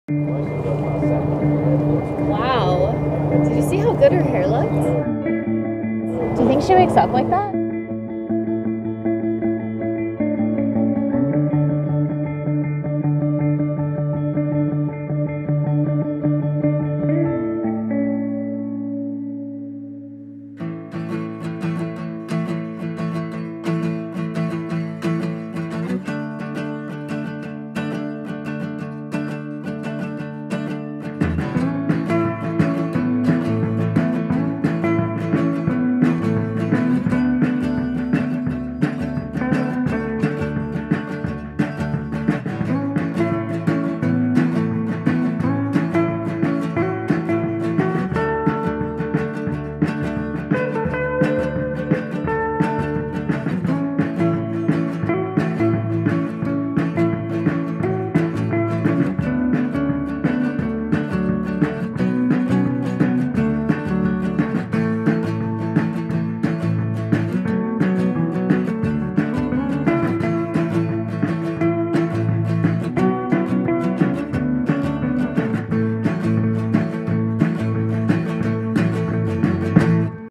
The Ultimate Bed Head | Hair Tutorial | Free People
An effortless look that works for any occasion, from day to night. Learn how to get that perfectly tousled, just-rolled-out-of-bed look with just a few easy steps!